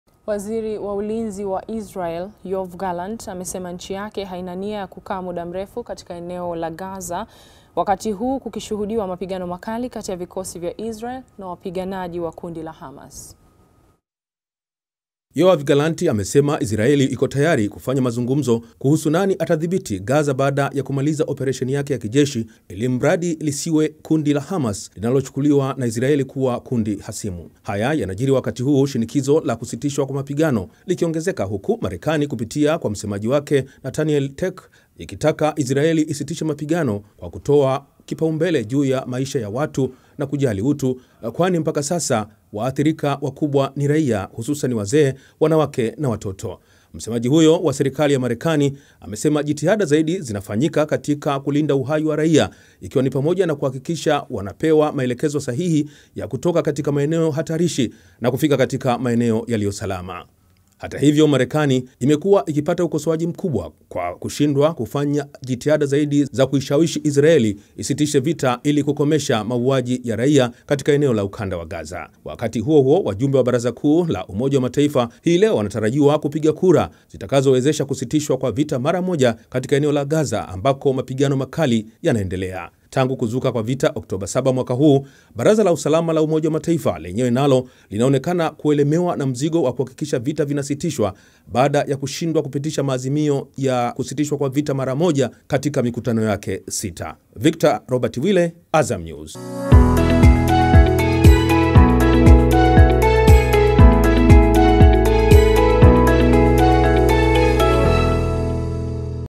Israel yasema haina nia wa kukaa muda mrefu Gaza
Waziri wa Ulinzi wa Israel, Yoav Gallant amesema kuwa nchi yake haina nia ya kukaa muda mrefu katika eneo la Ukanda wa Gaza wakati huu kukishuhudiwa mapigano makali kati ya vikosi vya Israel na wapiganaji wa kundi la Hamas.

Imehaririwa na Onike Masayanyika, John Mbalamwezi